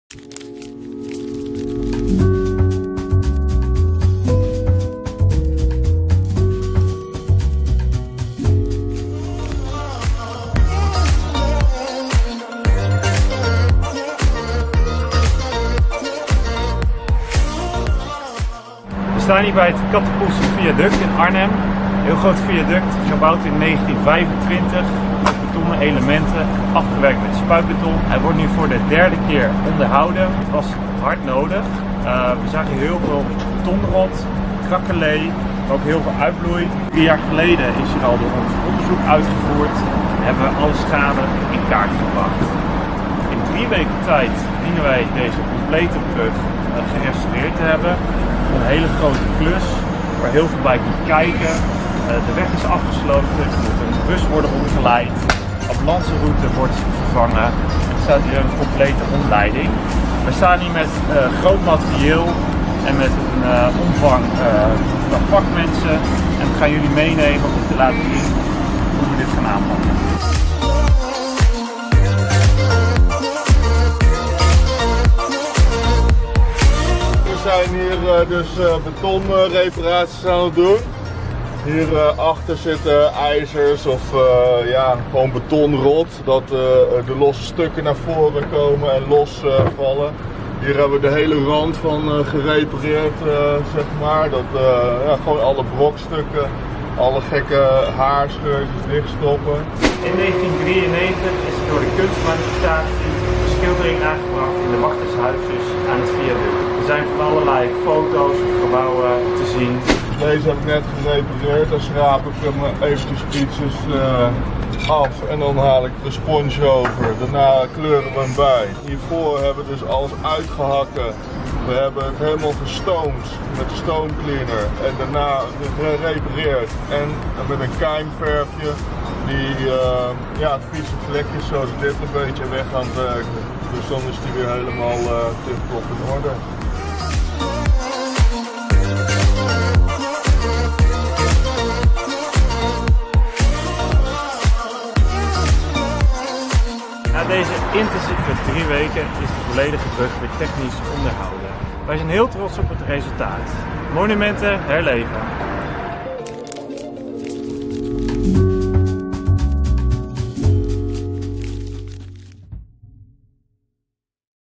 Restauratie van het Cattepoelse Viaduct in Arnhem
MOOI PROJECT! 💪🏻📽

Na 3 jaar voorbereiden zijn wij druk bezig om een mooi project af te ronden. Het Cattepoelse viaduct in Arnhem wordt volledig gerestaureerd zodat deze er weer een aantal jaar tegenaan kan.

We kunnen dit mooie project realiseren door de samenwerking met de Gemeente Arnhem, Connexxion, Takkenkamp Gevelonderhoud en Van Amerongen.